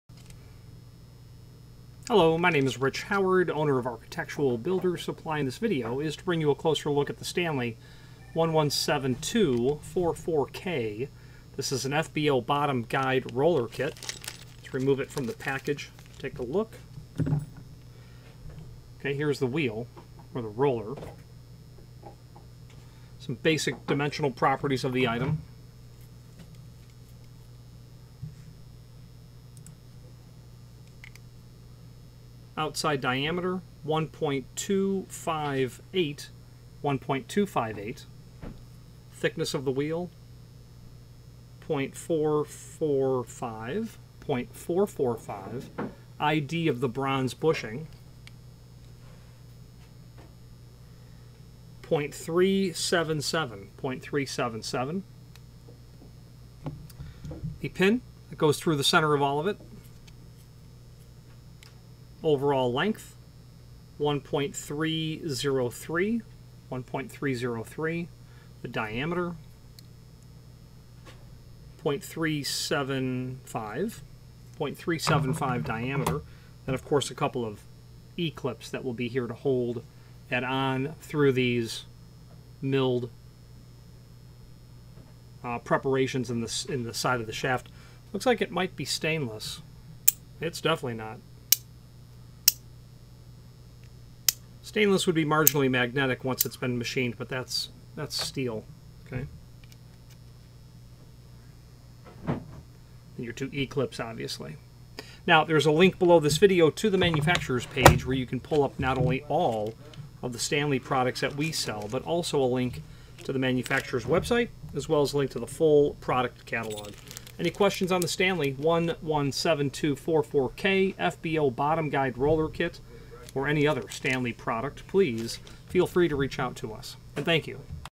Stanley 117244K FBO Bottom Guide Roller Kit
This video from absupply.net is to bring you a closer look at the Stanley 117244K FBO Bottom Guide Roller Kit where we will visually review the item along with its dimensional properties and its intended uses.